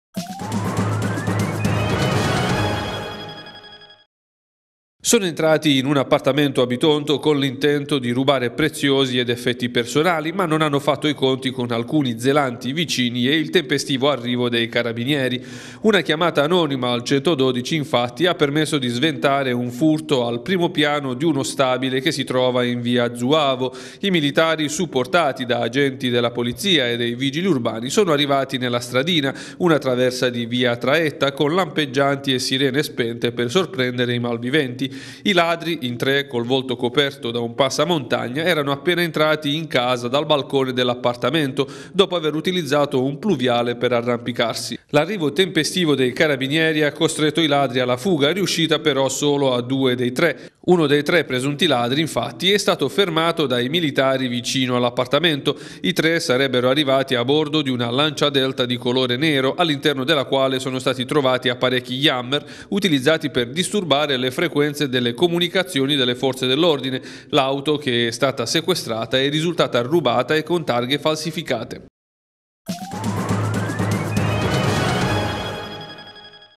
Furto in appartamento a Bitonto: un arresto
Sono entrati in un appartamento, a Bitonto, con l’intento di rubare preziosi ed effetti personali, ma non avevano fatto i conti con alcuni zelanti vicini e il tempestivo arrivo dei Carabinieri. Una chiamata anonima al 112, infatti, ha permesso di sventare un furto al primo piano di uno stabile che si trova in via Zuavo. I militari, supportati da agenti della Polizia e dei Vigili Urbani, sono arrivati nella stradina, una traversa di via Traetta, con lampeggianti e sirene spente per sorprendere i malviventi. I ladri, in tre, col volto coperto da passamontagna, erano appena entrati in casa dal balcone dell’appartamento, dopo aver utilizzato un pluviale per arrampicarsi. L’arrivo tempestivo dei Carabinieri ha costretto i ladri alla fuga, riuscita però solo a due dei tre. Uno dei tre presunti ladri, infatti, è stato fermato dai militari vicino all’appartamento. I tre sarebbero arrivati a bordo di Lancia Delta di colore nero, all'interno della quale sono stati trovati apparecchi jammer, utilizzati per disturbare le frequenze delle comunicazioni delle Forze dell’Ordine. L'auto, che è stata sequestrata, è risultata rubata e con targhe falsificate.